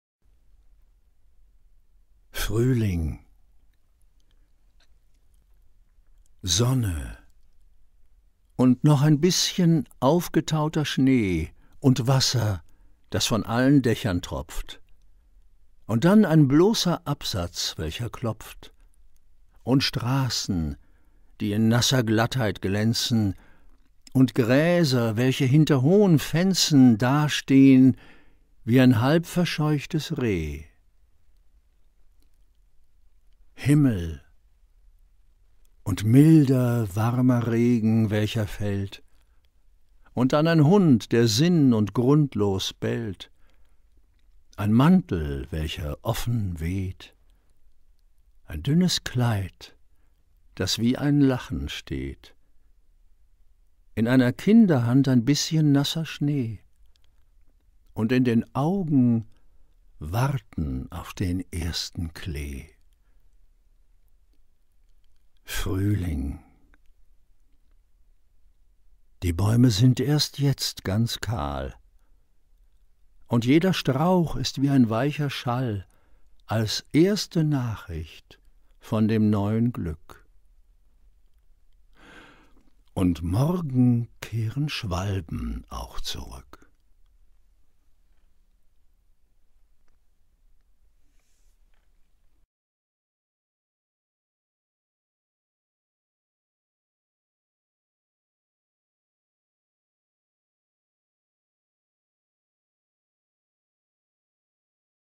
Selma Meerbaum-Eisinger „Frühling“
Text:

Sonne. Und noch ein bißchen aufgetauter Schnee
und Wasser, das von allen Dächern tropft,
und dann ein bloßer Absatz, welcher klopft,
und Straßen, die in nasser Glattheit glänzen,
und Gräser, welche hinter hohen Fenzen
dastehen, wie ein halbverscheuchtes Reh...

Himmel. Und milder, warmer Regen, welcher fällt,
und dann ein Hund, der sinn- und grundlos bellt,
ein Mantel, welcher offen weht,
ein dünnes Kleid, das wie ein Lachen steht,
in einer Kinderhand ein bißchen nasser Schnee
und in den Augen Warten auf den ersten Klee...

Frühling. Die Bäume sind erst jetzt ganz kahl
und jeder Strauch ist wie ein weicher Schall
als erste Nachricht von dem neuen Glück.
Und morgen kehren Schwalben auch zurück.

Bilder: Fotocollage